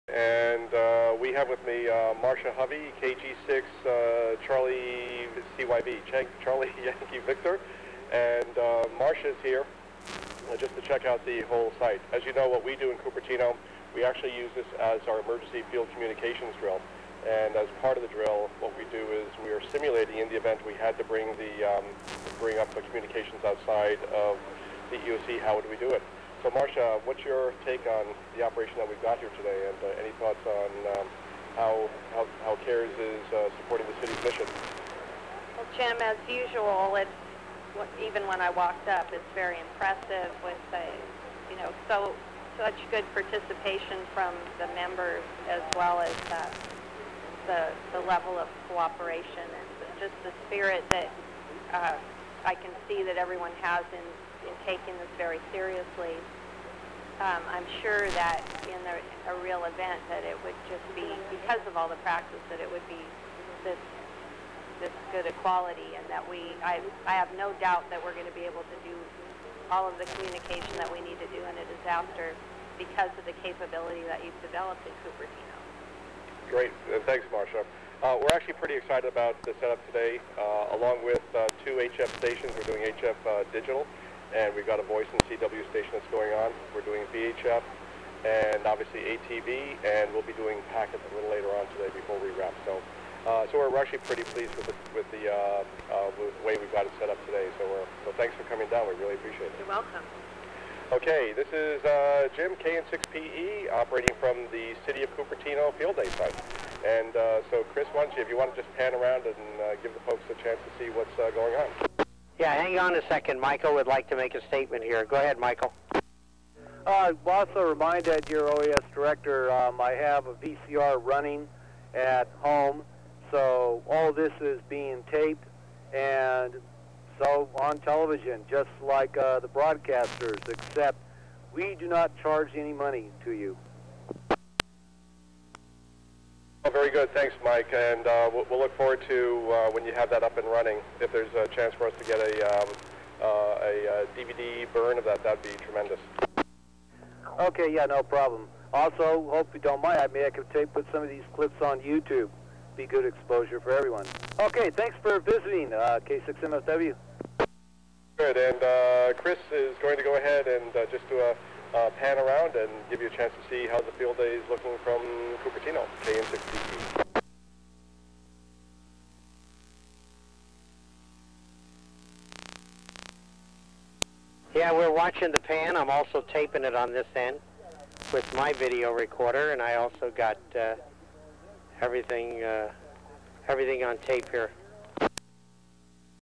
KN6PE interviews Cupertino OES director on K6BEN ATV
Jim KN6PE interviews Cupertino OES director Marsha Hovey KG6CYV at Cupertino Amateur Radio Emergency Services (CARES) Field Day site on June 28, 2008.

This is over-the-air transmissions just like commercial broadcast television. Video was uplinked on 1255MHz and re-transmitted throughout south San Francisco Bay Area (Silicon Valley) on 421.25MHz which is same a cable (CATV) channel 57. Audio uplink was done using regular 2-way transmission on 145.510MHz (2meter simplex).

Video transmissions were through the K6BEN amateur television (ATV) station repeater. CARES transmitted to K6BEN (about 10 miles away) using a VideoLynx Z23B (2 watts RF power) and a high gain loop yagi raised 30 feet to clear buildings. This reception was taped on a VCR located elsewhere in Silicon Valley (about 11 miles away), and edited for this clip to youtube.

See CARES 2008 Field Day site for report with photos on their setup,